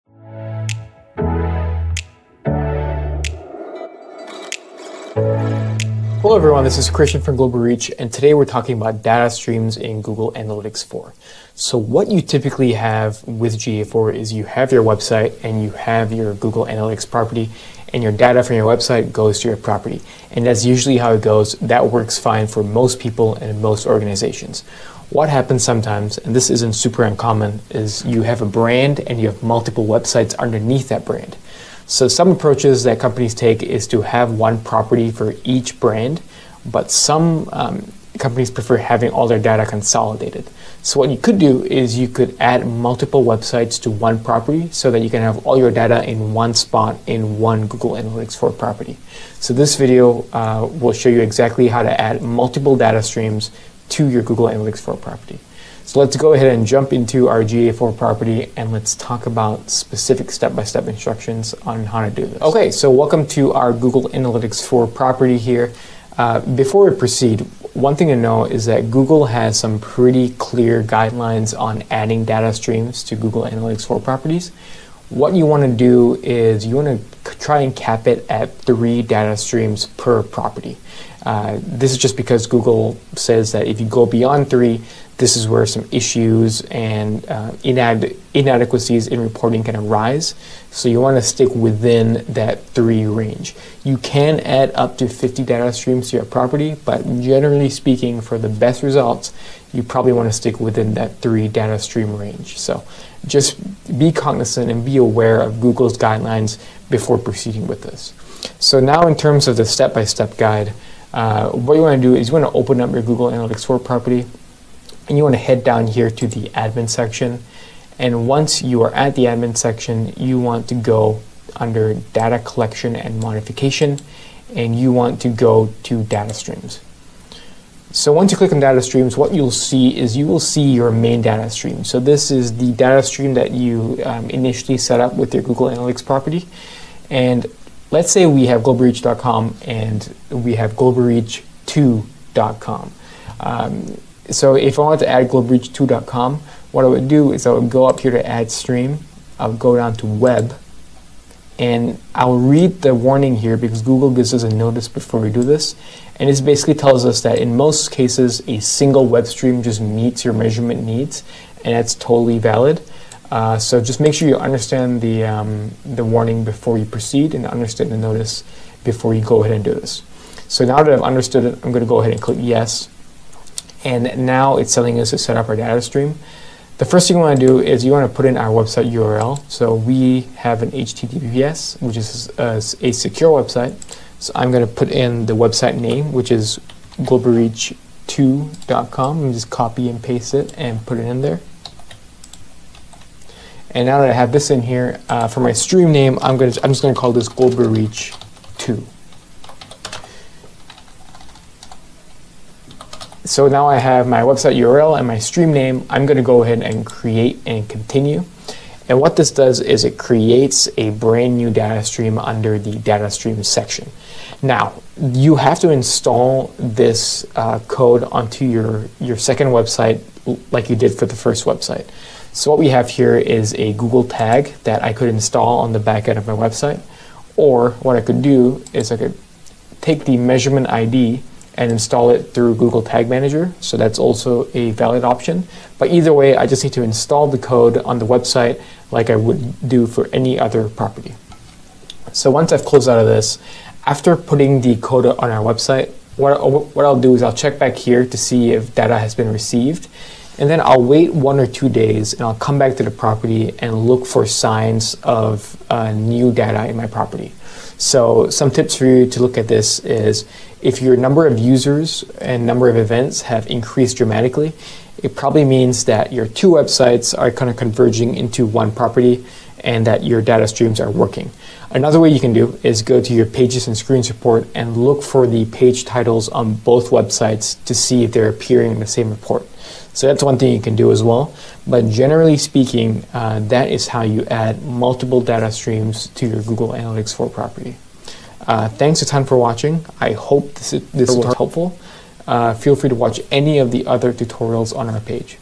How to add multiple websites to Google Analytics 4 | Full Data Streams Tutorial
The data stream functionality in Google Analytics 4 allows you to track multiple domains under one property. While it can be tempting to add all of your domains, Google has some particular best practices when dealing with multiple web streams. This video breaks it down in a step-by-step guide.

0:00 Introducing Data Streams
1:02 Explaining Google's Data Stream Guidelines
1:48 Step-by-Step Guide